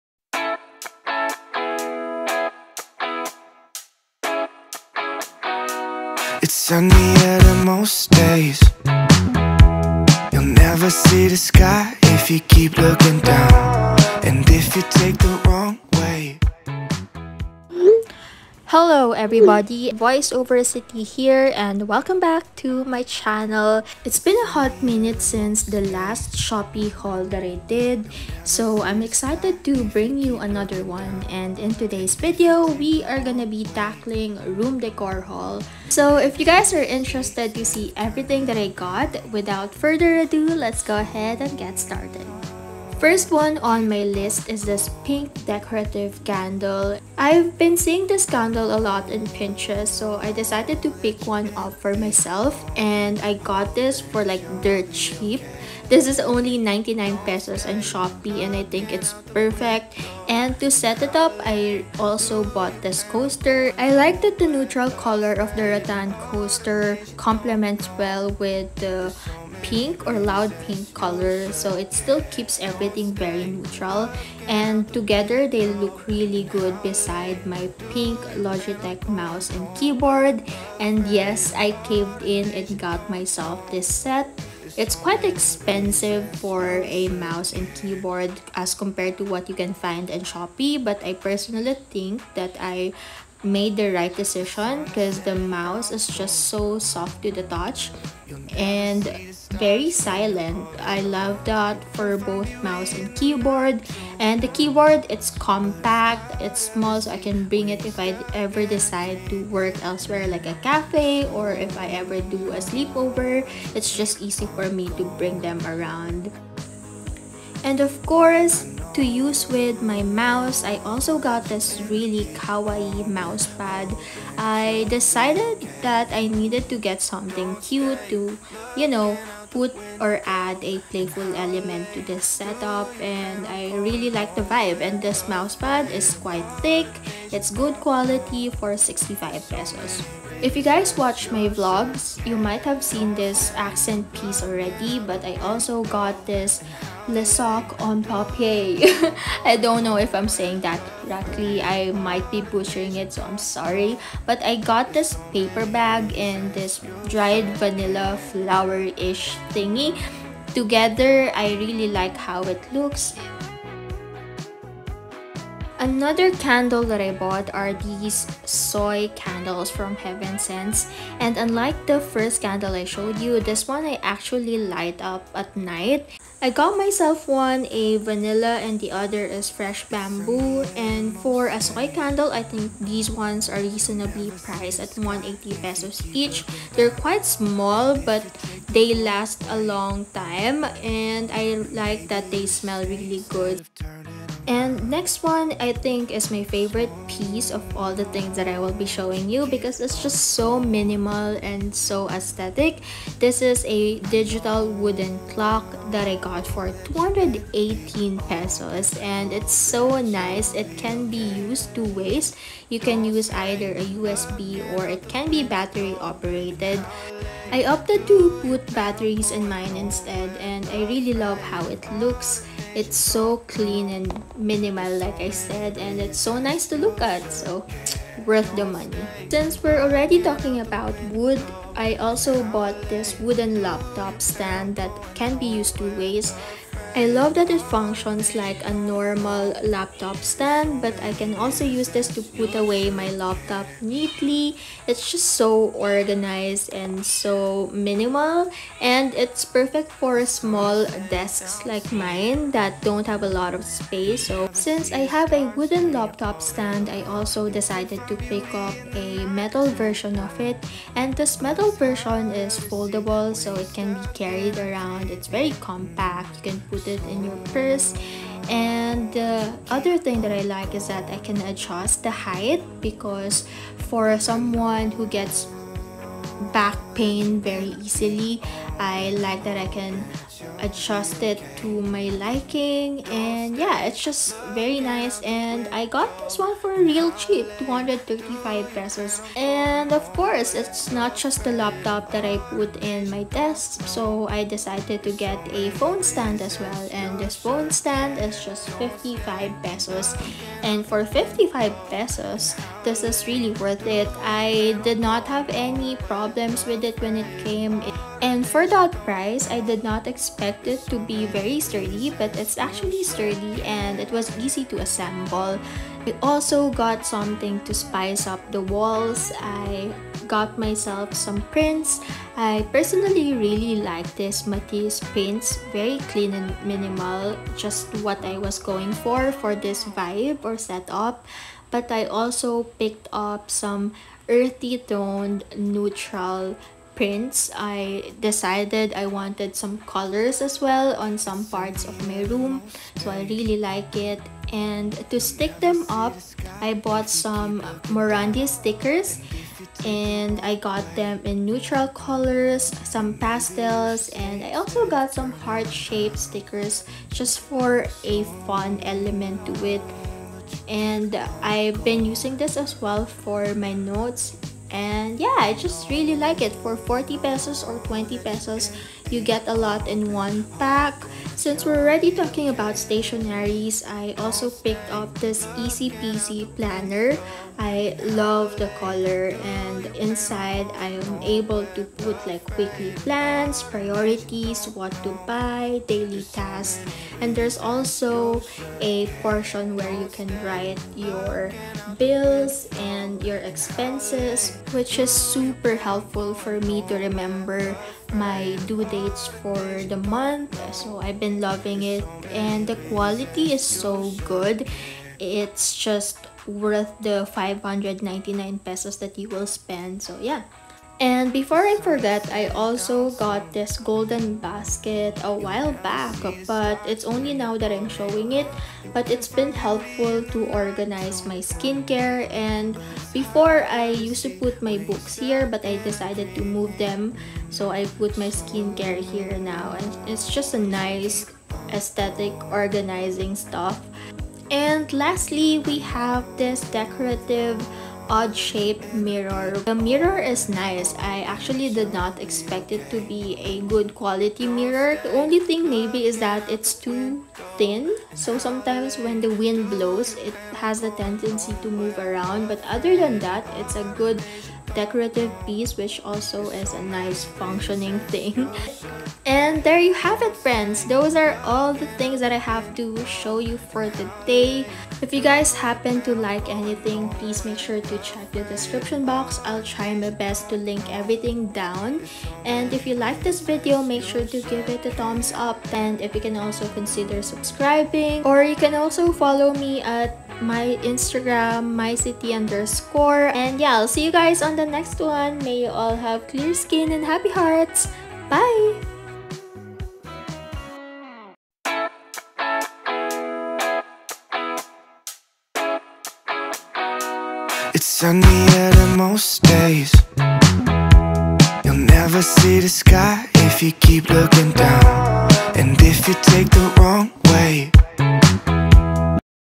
SHOPEE Aesthetic Room Decor Haul 2021 ⚡️ my best finds yet!! | Sittie Saheda
Watch in HD! SHOPEE Aesthetic Room Decor Haul 2021

It's been a hot minute since the last Shopee Haul that I did so I'm coming back at ya today with an aesthetic room decor haul of my favorite inexpensive/cheap/budget (however you wanna call it haha) decor pieces to date. :)